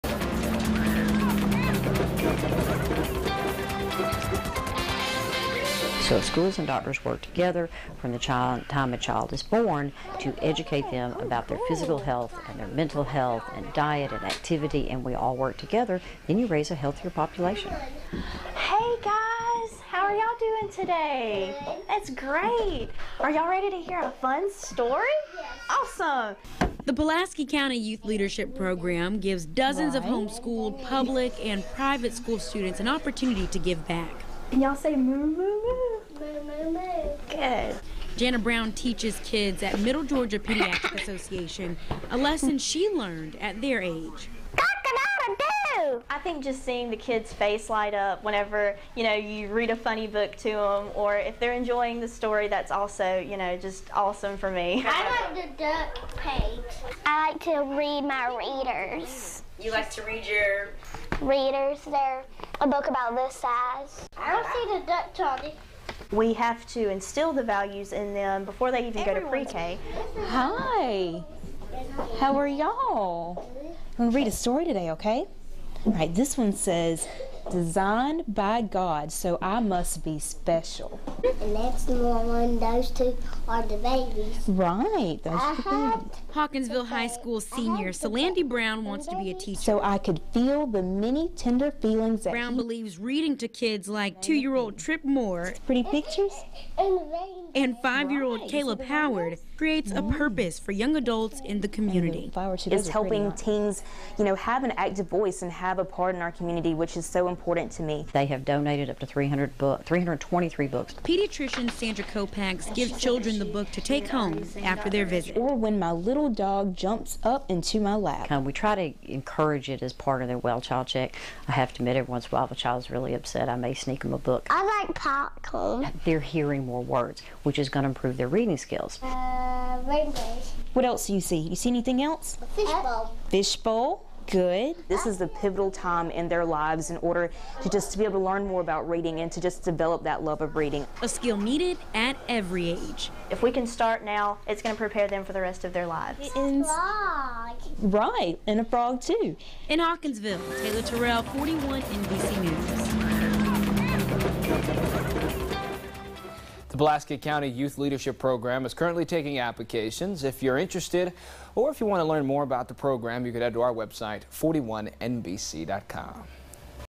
Whats Right With Our Schools
Pulaski County Students helping children with check ups